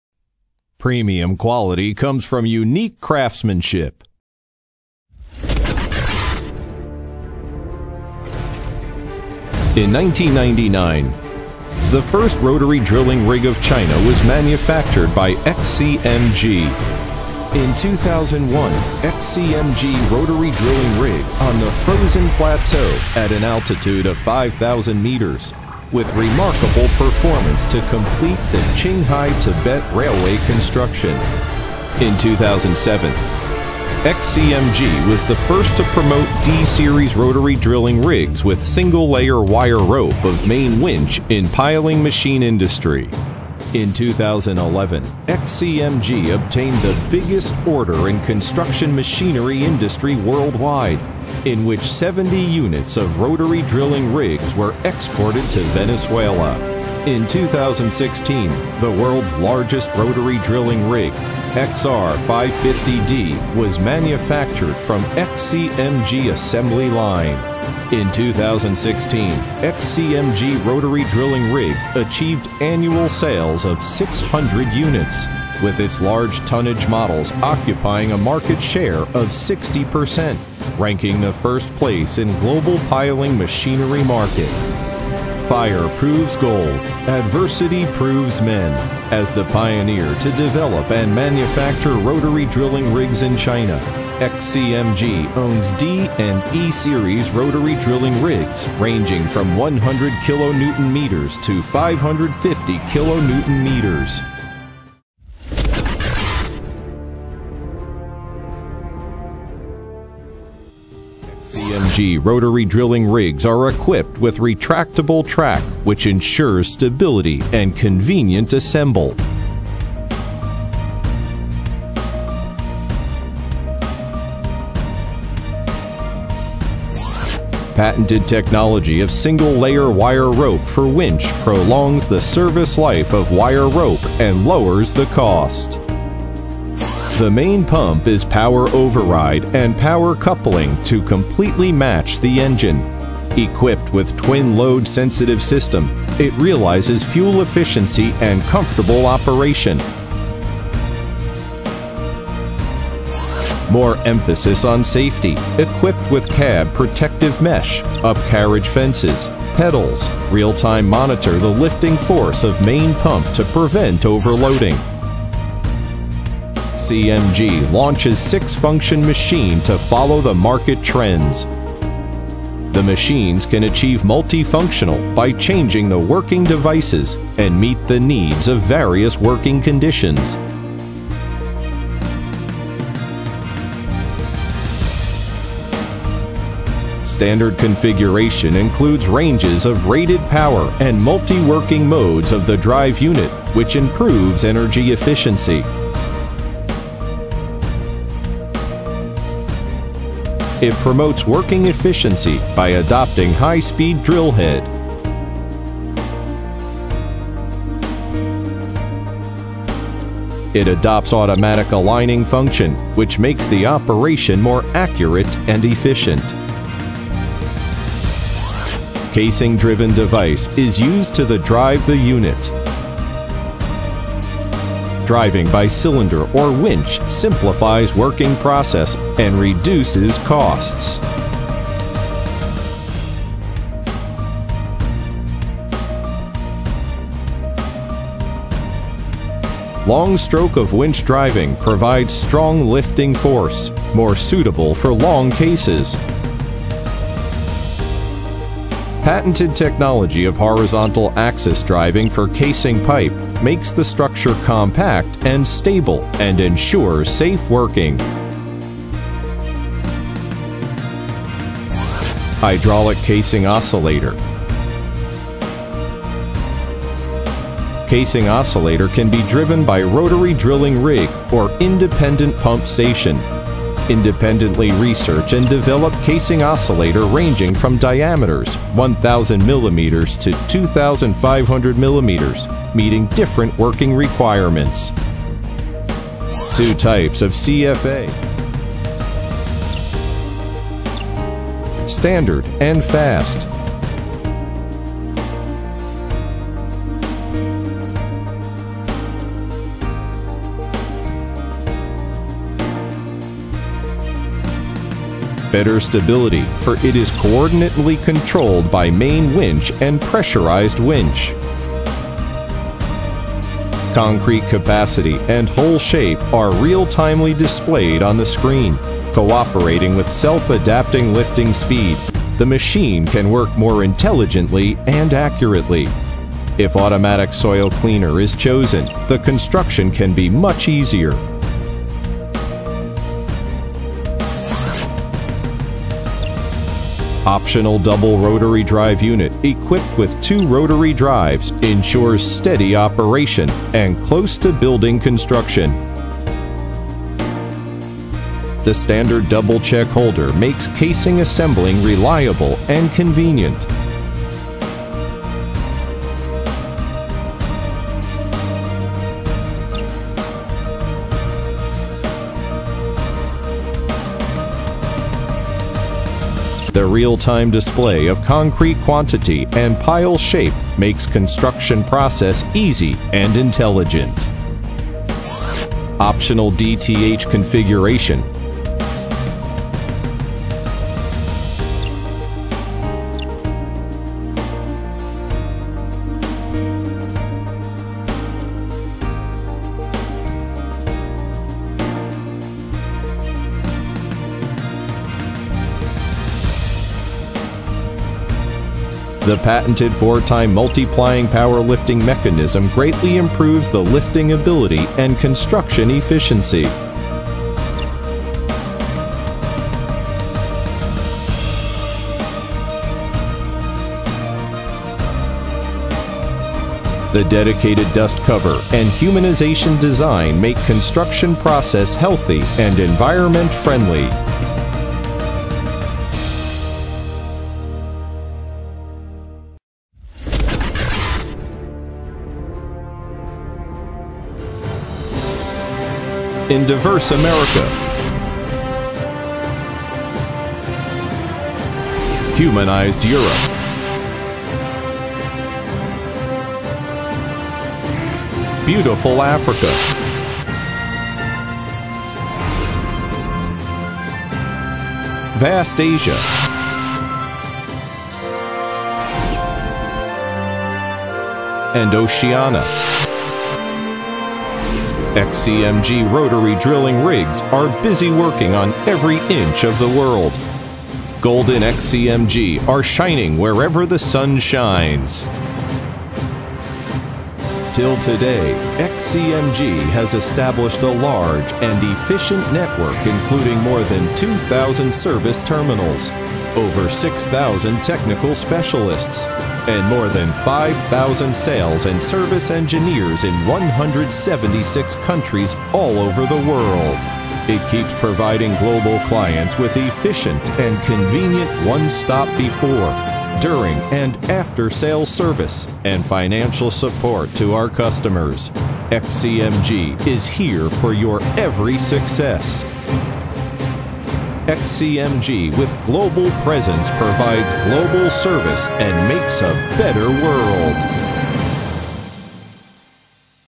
XCMG Rotary Drilling Rig for Your Success
The technology level of XCMG rotary drilling rigs is at its peak ever since 1999 when XCMG rolled out the first drilling rig in China. Coming along with the best quality and one-stop integrated services, our drilling rigs have been setting foot all over the globe.